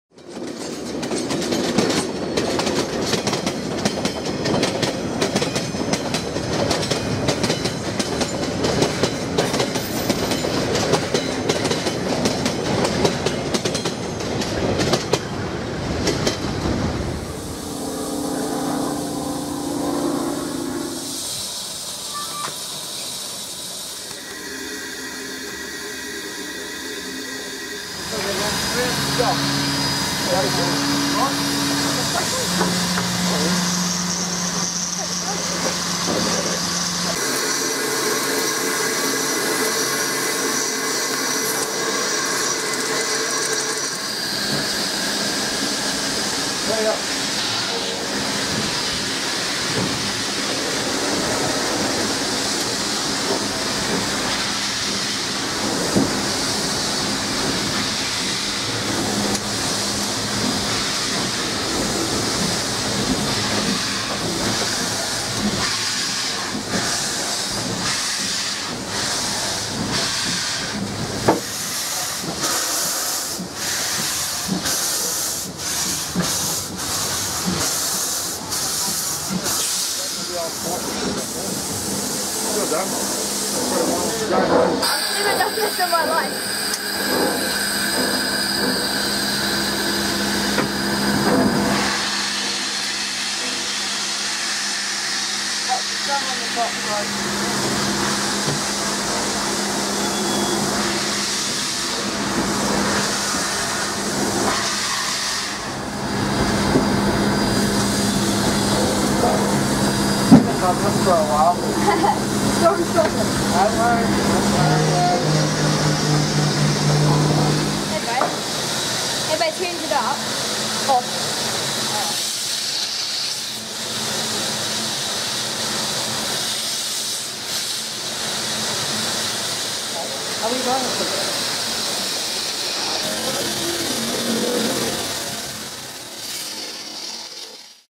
KA942 at Mainline steam
KA942 shunting around the Mainline Steam Yard in Christchurch.